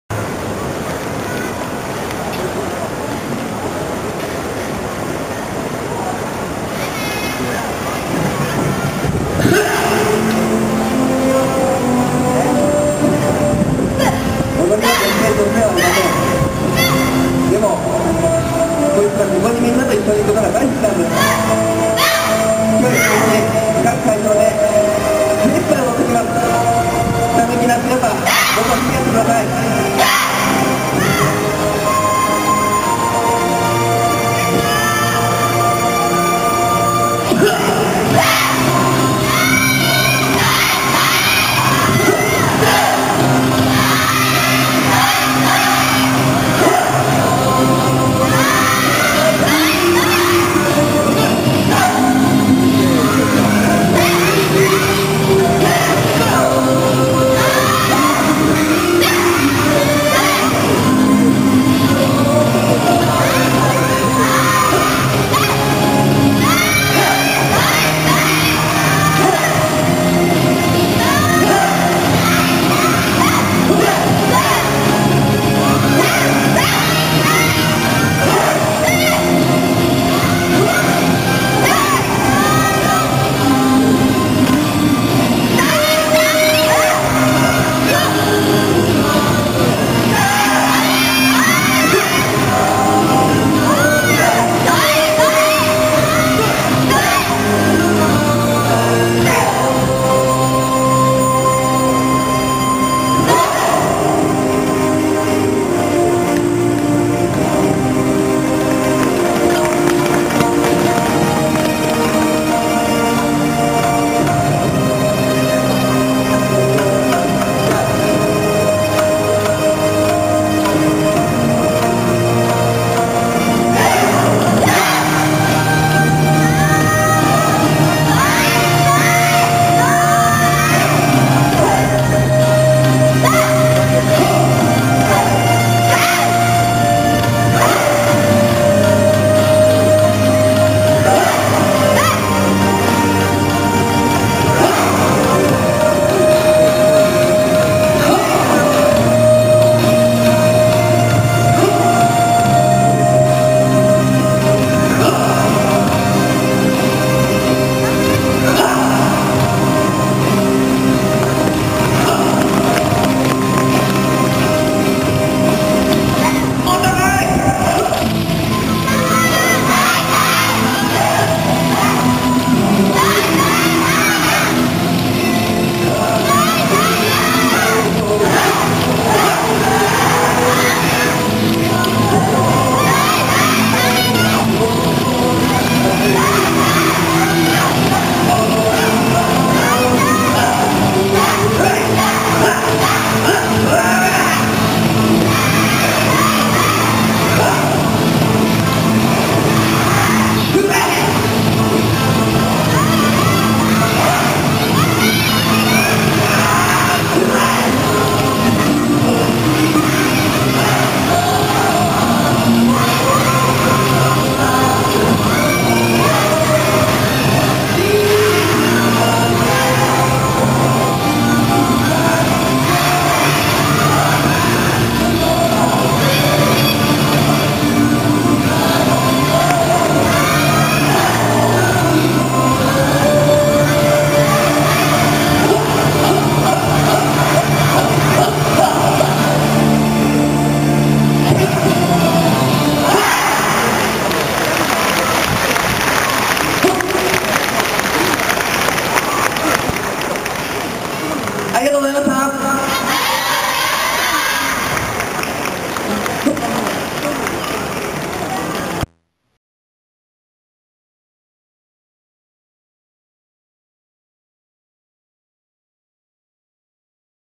襲雷舞踊団（ドリームよさこい・2010・丸の内仲通りなど）
2010年10月31日　ドリームよさこい　パレットタウン・石と光の広場・丸の内仲通り　非常に若々しいチームながら迫力も満点です。
Young Yosakoi Team 'Syuurai Buyoudan'